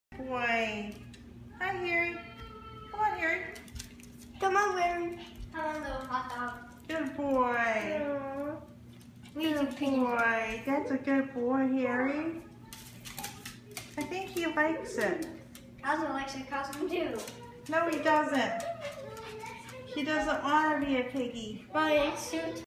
Harry the Yorkie and Cosmo the Maltese - Halloween 2015
Our dogs trying out their Halloween costumes!